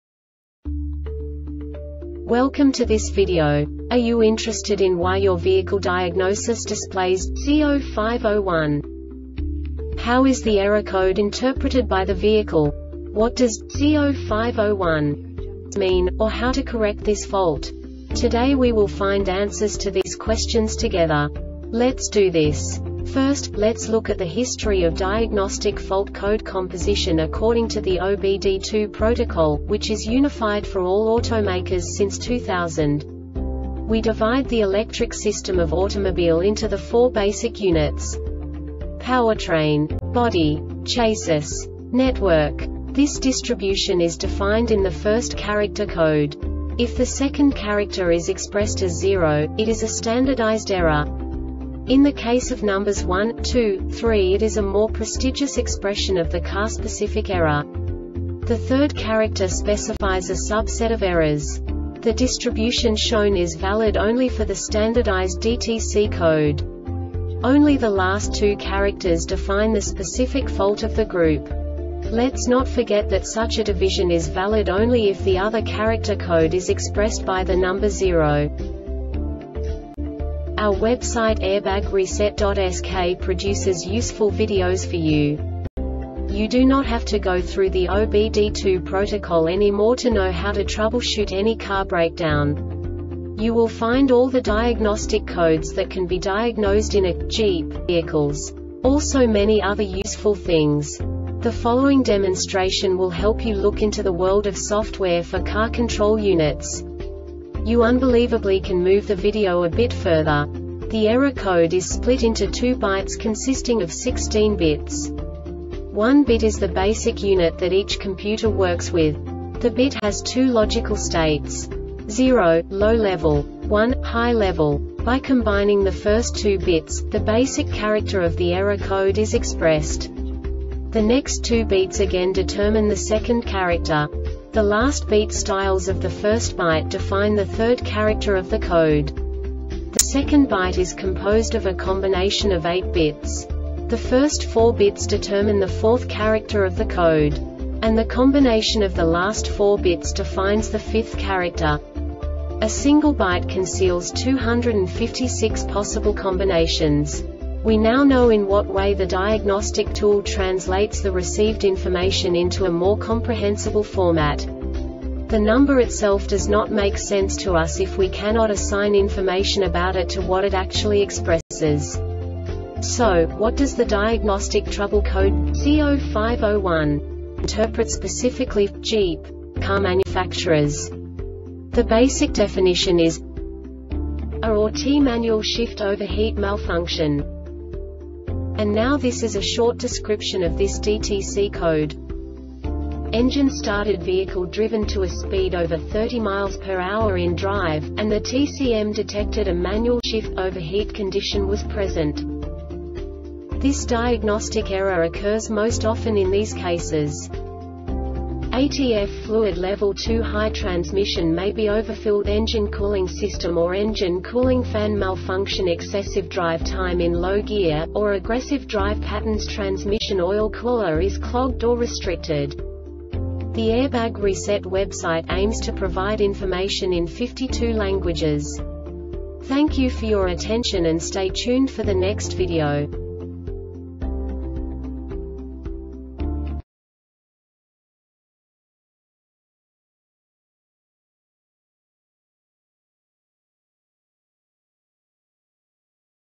DTC Jeep C0501 Short Explanation
Contents:
0:21 Basic DTC analysis according to OBD2 protocol standard.
1:48 Insight into programming
2:58 A diagnostically understandable description of the Diagnostic Trouble Code
3:05 Basic error definition

Make:
Jeep
Code:
C0501
Definition:
A or T Manual Shift Overheat Malfunction
Description:
Engine started;vehicle driven to a speed over 30 mph in Drive,and the TCM detected A Manual Shift Overheat condition was present.
Cause:
ATF fluid level too high (transmission may be overfilled)Engine Cooling System or engine cooling fan malfunctionExcessive drive time in low gear,or aggressive drive patternsTransmission oil cooler is clogged or restricted